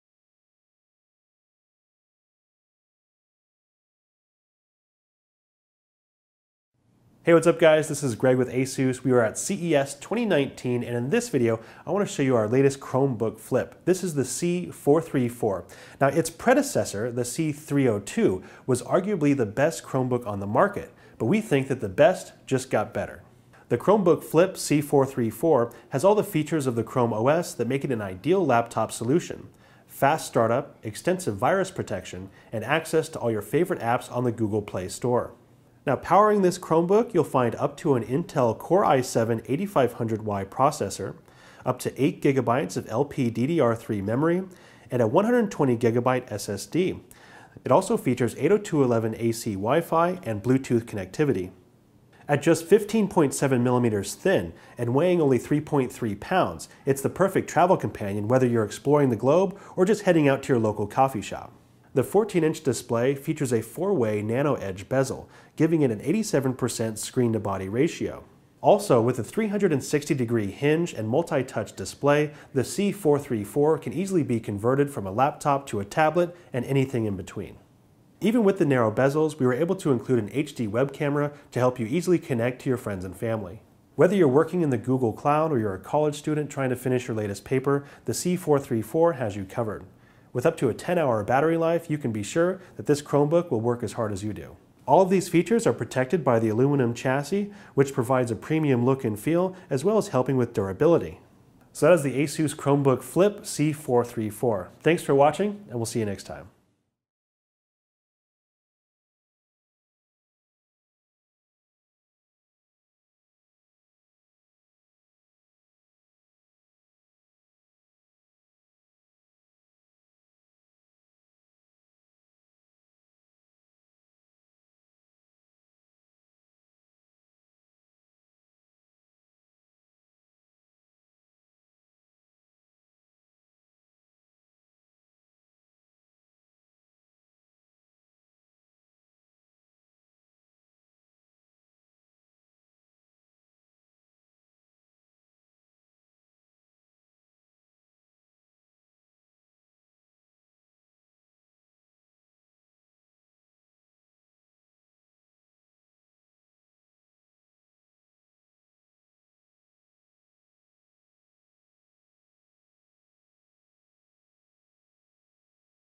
Asus Chromebook Flip C434 2 in 1 Laptop
Asus Chromebook Flip C434 2 In 1 Laptop, 14" Touchscreen FHD 4-Way NanoEdge, Intel Core M3-8100Y Processor, 4GB RAM, 64GB eMMC Storage, All-Metal Body, Backlit KB, Silver, Chrome OS, C434TA-DSM4T

• Get 3 months of Disney+ with purchase of new Chromebook from 11/25/19 to 1/31/2019. See Google’s official event site for details.
• 14 inch touchscreen full HD 1920x1080 4-way NanoEdge Display featuring ultra-narrow bezels (5mm thin) around each side of the display that allows for a 14 inch screen to fit in the body of a 13 inch Laptop footprint
• The full HD display has a durable 360 degree hinge that can be used to flip the touchscreen display to tent, stand, and tablet mode
• Powered by the Intel Core m3-8100y Processor (4M Cache, up to 3. 4 GHz) for super-fast and snappy performance. If you use a ton of tabs or run lots of apps, this has the power to get it all done with ease